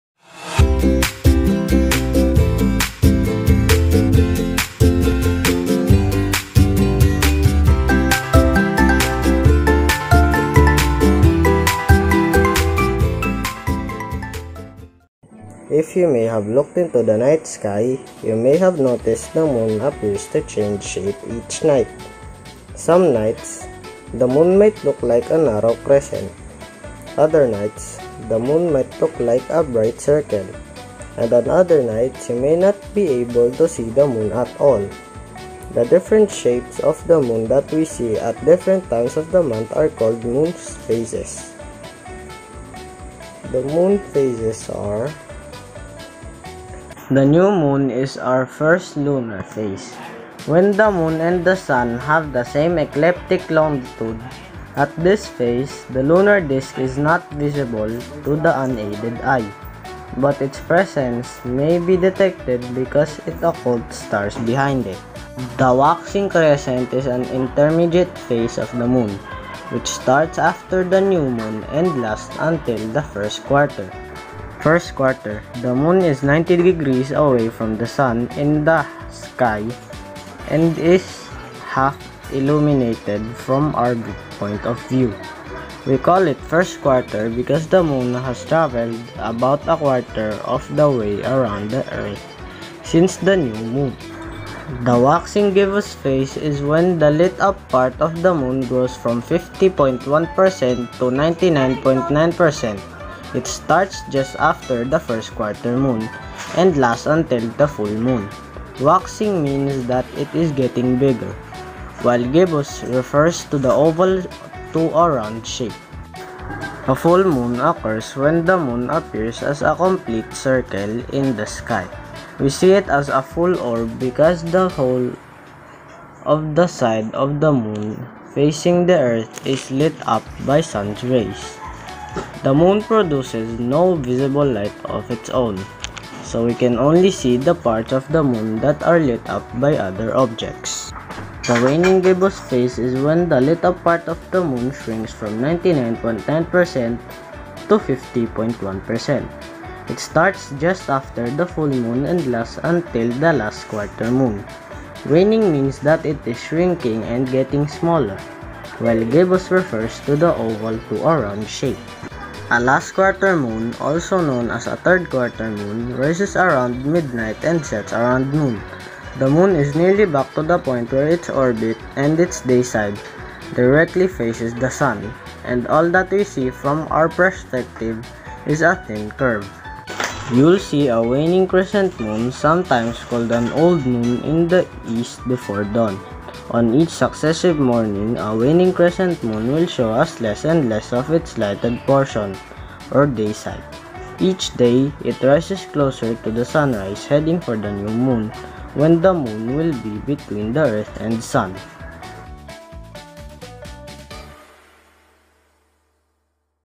MOON PHASES MINIATURE -Made of oreo biscuits on illustration board
Science 5
Integrative Performance Task

Learning competency: identify the different phases of the moon.

Role: to become a miniaturist

A miniaturist is a person making miniature objects. They make small and tiny objects as a resemblance to the bigger objects.

Task: Create and label the different phases of the moon using the oreo biscuits on illustration board.